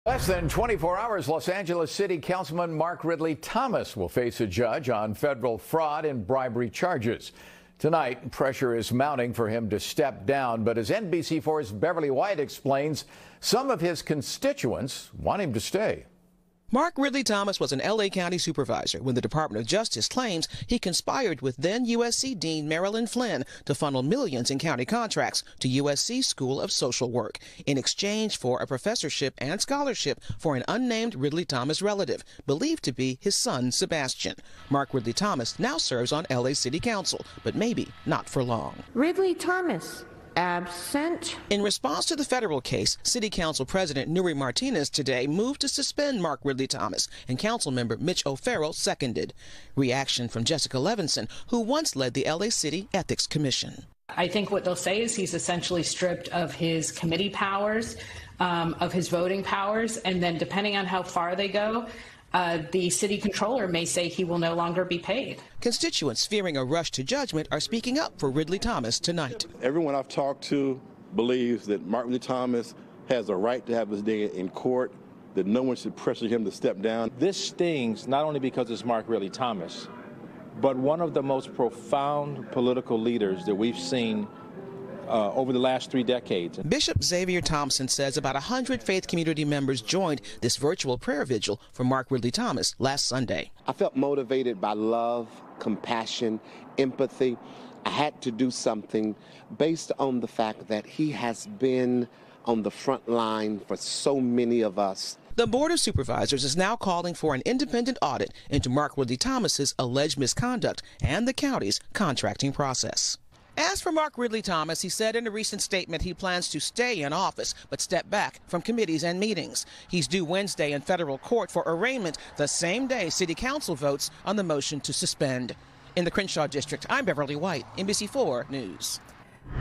Mark Ridley-Thomas to Face Federal Charges in Bribery and Fraud Case | NBCLA
Pressure is mounting at LA City Hall for Councilmember Mark Ridley-Thomas to step down, following his indictment last week on federal bribery and fraud charges. Beverly White reports for the NBC4 News on Tuesday, Oct. 19, 2021.

About NBCLA